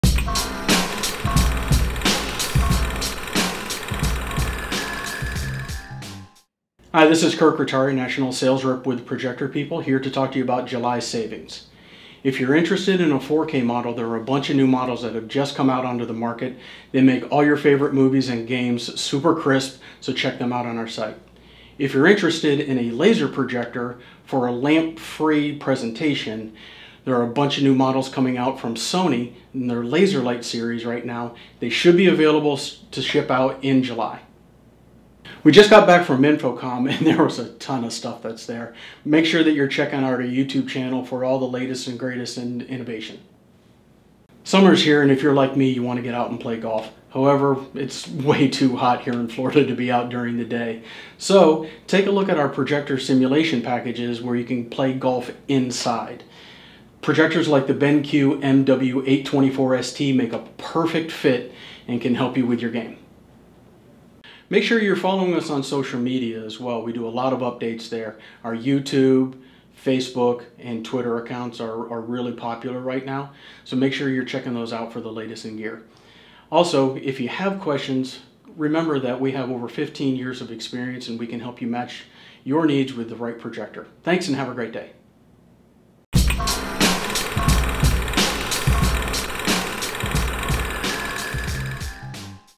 Projector Deals for July 2017 from Projector People!
Projector expert Kirk Ritari shares information about our current promotions and current sales for July 2017. Kirk goes over new affordable 4K, InfoComm and the best projector pick for your golf simulator!